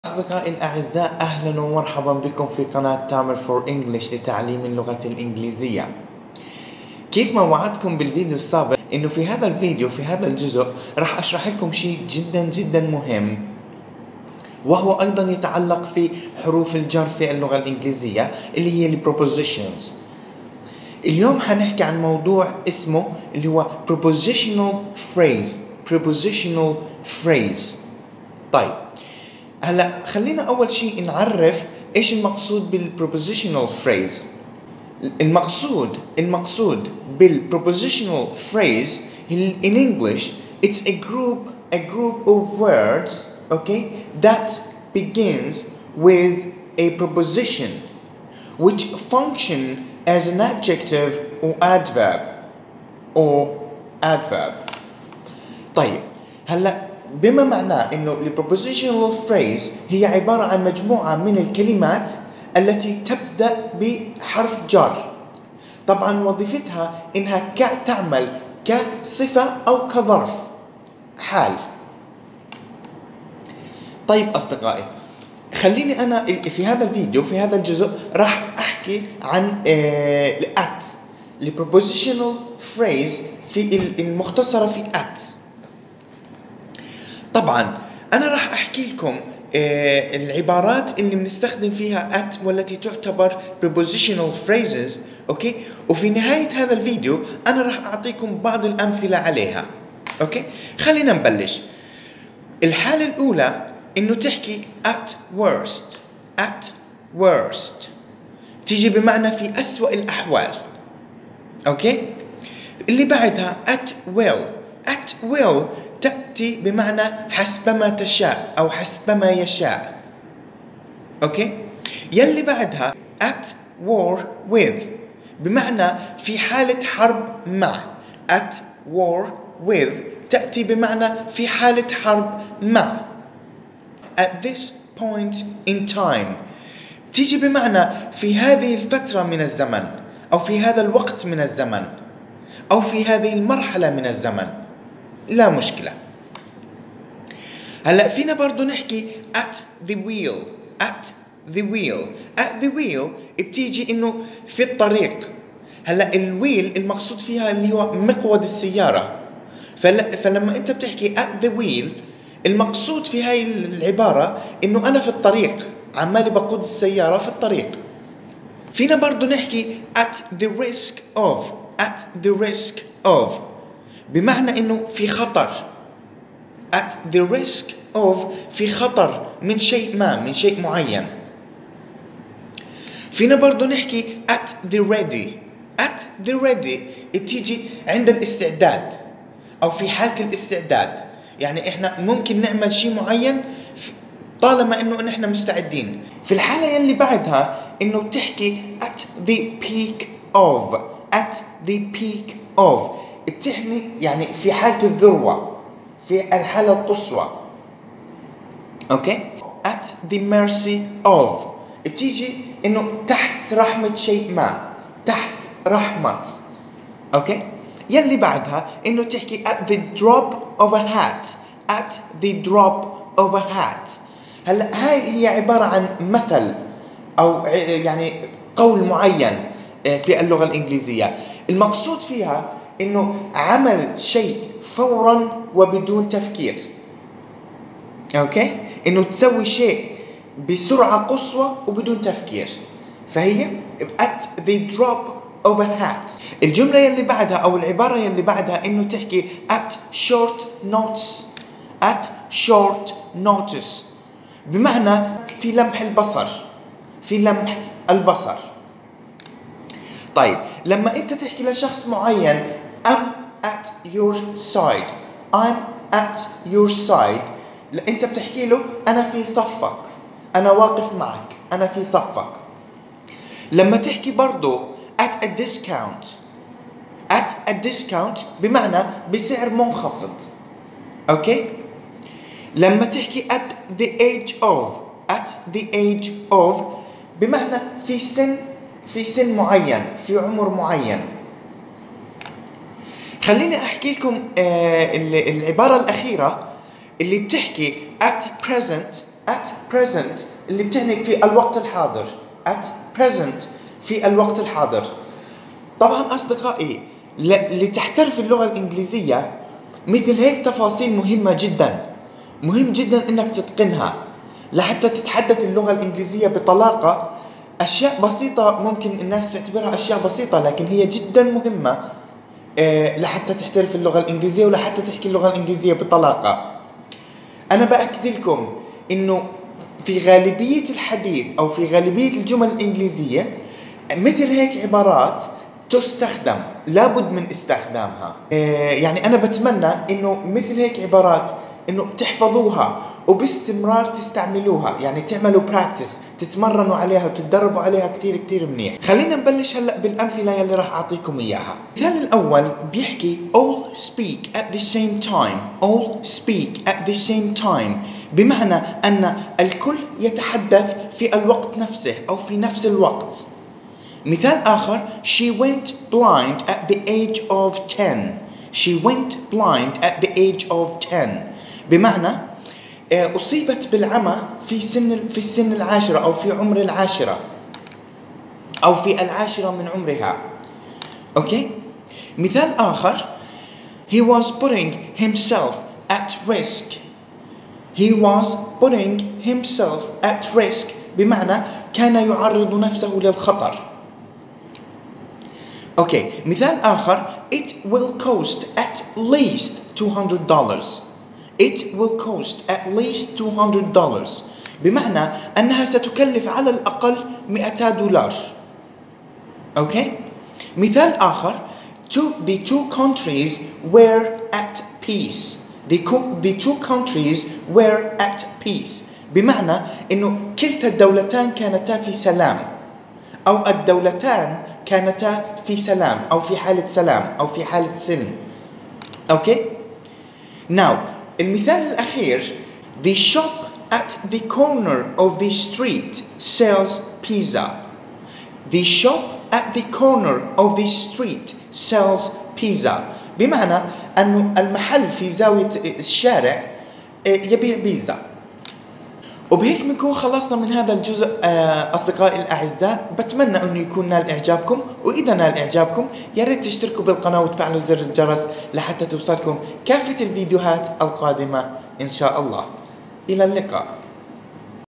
إحترف اللغة الانجليزية بمشاهدة هذا الفيديو/درس القواعد/prepositional phrase/ عباراة الجر AT
نعرض لكم في هذا الجزء شرح لعبارة الجر AT مع العديد من الامثلة عليها
أفضل الشروحات لقواعد وأساسيات اللغة الانجليزية للمبتدئين